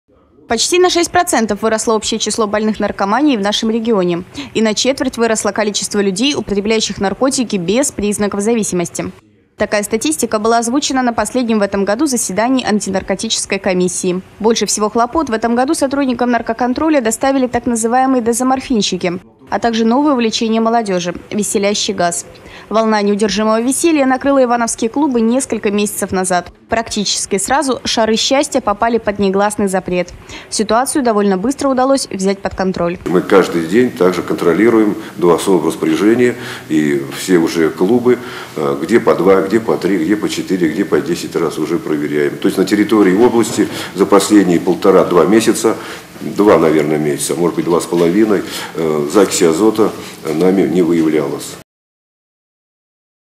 Шаров счастья больше нет
Наркоконтроль за последние 2 месяца не выявил случаев злоупотребления закисью азота